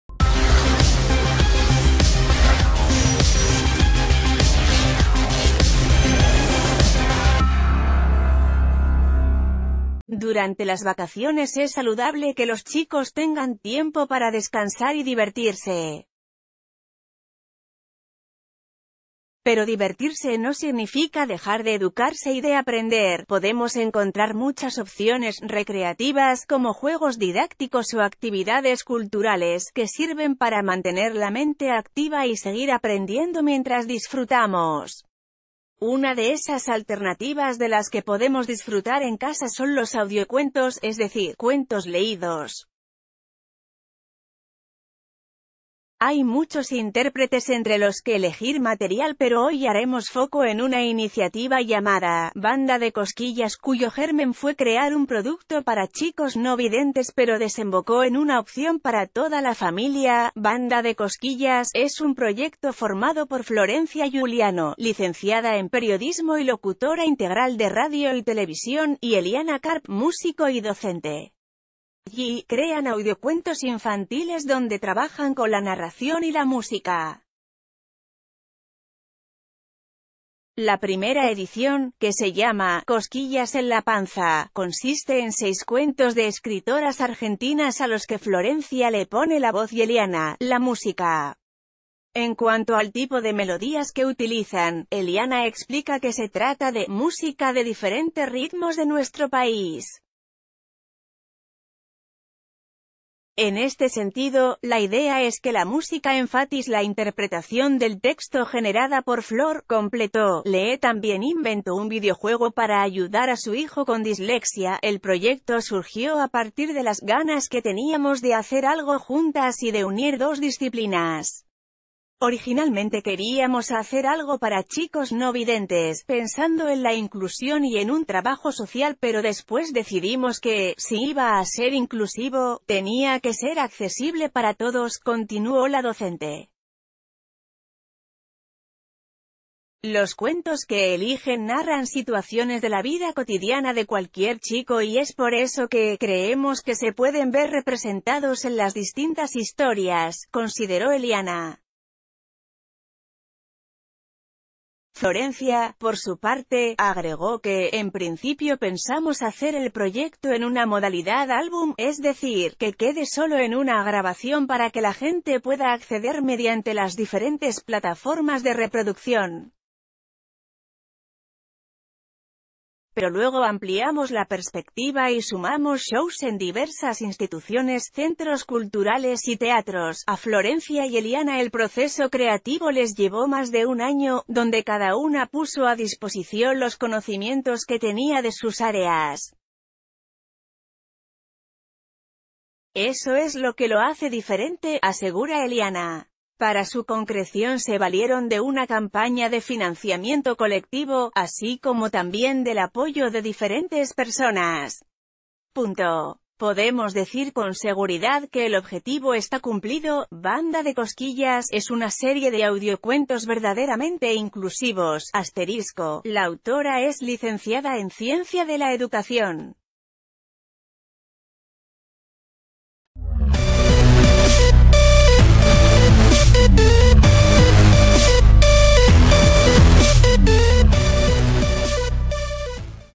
Audiocuentos: una opción para que los chicos se diviertan y aprendan en las vacaciones
Durante las vacaciones es saludable que los chicos tengan tiempo para descansar y divertirse. Pero divertirse no significa dejar de educarse y de aprender: podemos encontrar muchas opciones recreativas (como juegos didácticos o actividades culturales) que sirven para mantener la mente activa y segui...